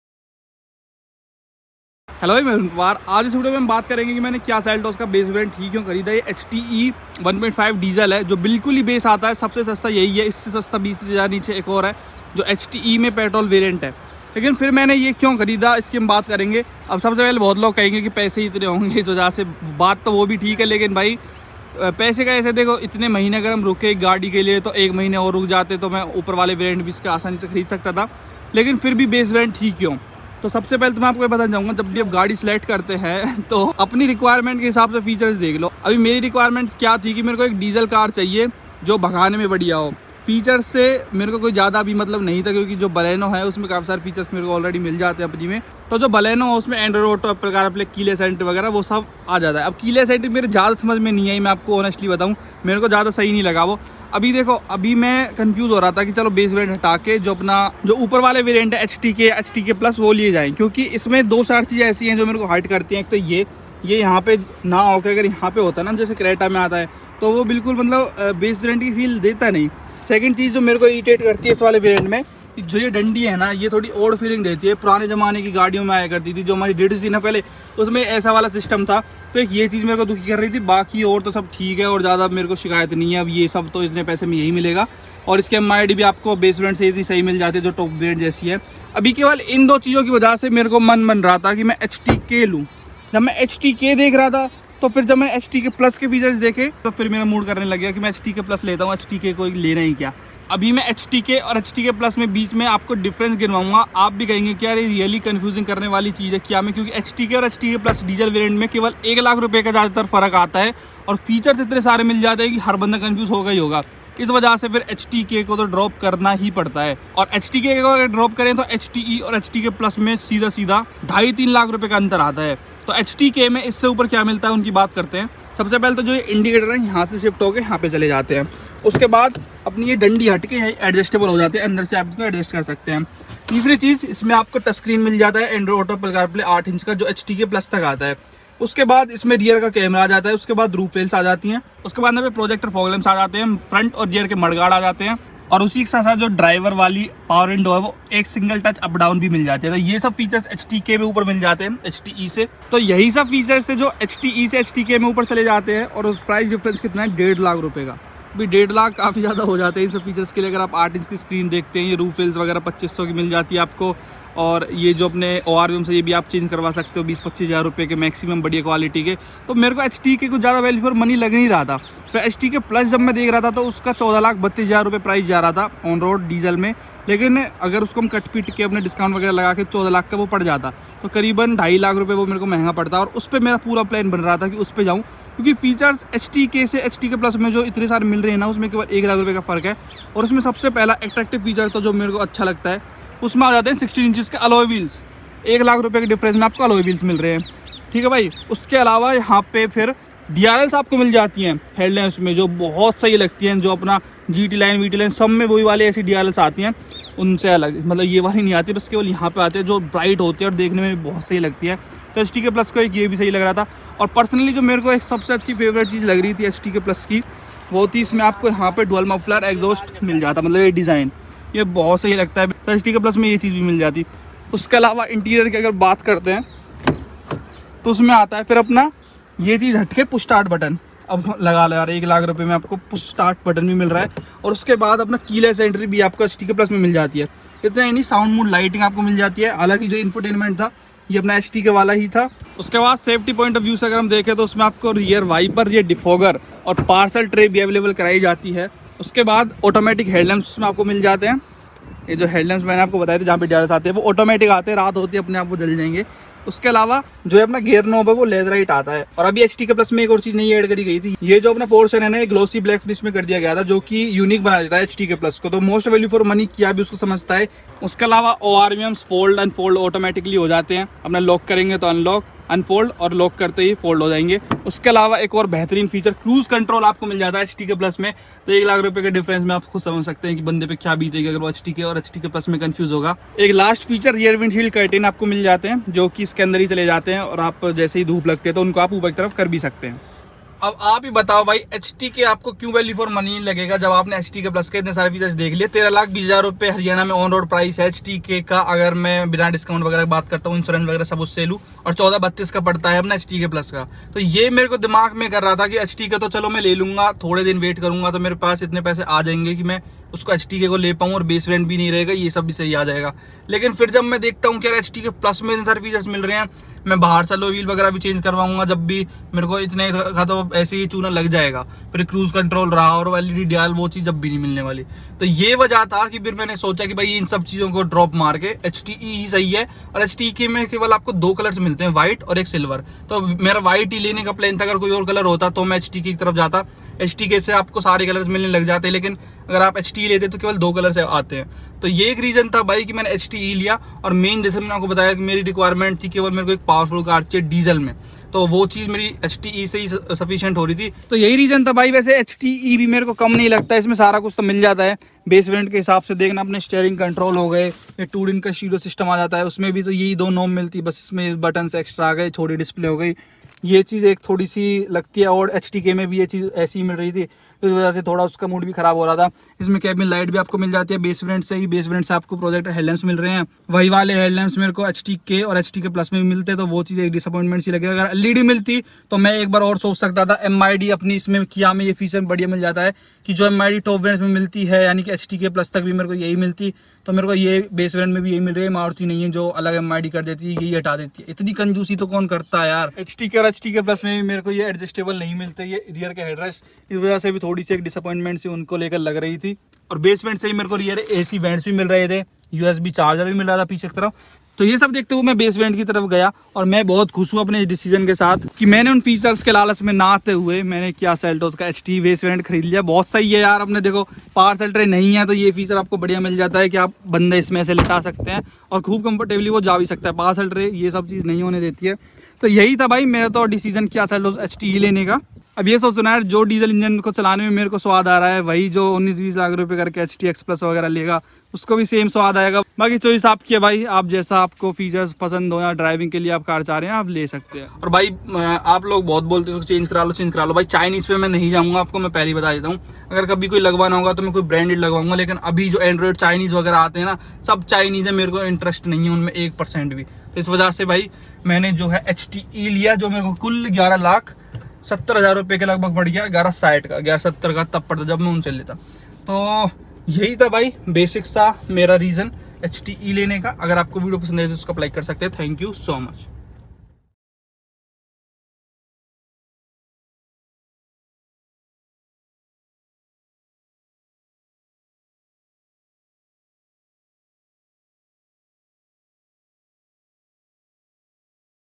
Why I Bought BASE MODEL of Kia Seltos? HTE 2020 Diesel | Top Reasons
About Me:
Hey, I am Arun from Palwal, Haryana (HR 30). Student of BCA and currently owning a Baleno 2019 Petrol and Kia Seltos Diesel 2020.